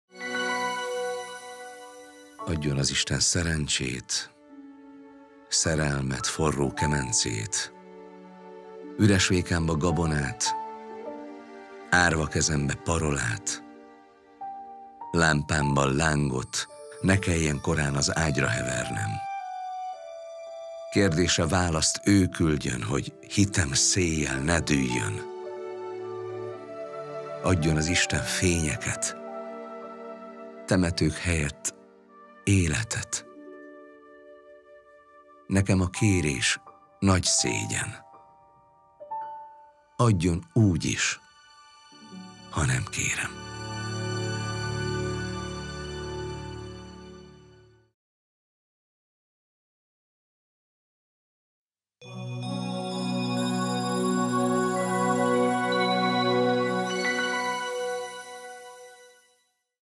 Nagy László: Adjon az Isten /Vers mindegy kinek (Lutter Imre versfilmje)
szerző: Nagy László
cím: Adjon az Isten
előadó: Lutter Imre

Adjon az Isten
szerencsét,
szerelmet, forró
kemencét,
üres vékámba
gabonát,
árva kezembe
parolát,
lámpámba lángot,
ne kelljen
korán az ágyra hevernem,
kérdésre választ
ő küldjön,
hogy hitem széjjel
ne dűljön,
adjon az Isten
fényeket,
temetők helyett
életet -
nekem a kérés
nagy szégyen,
adjon úgyis, ha
nem kérem.